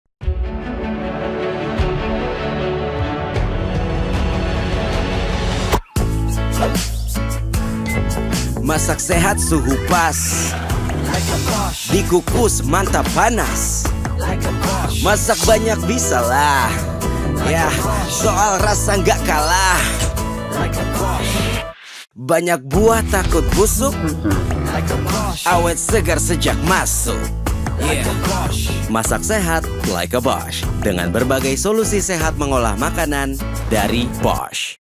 Cara Memasak Sehat dengan Sentuhan Bosch
Temukan bagaimana inovasi dapur Bosch dapat mengubah cara Anda memasak, membawa pengalaman memasak lebih sehat bersama Bosch. Saatnya ubah dapur Anda mulai memasak sehat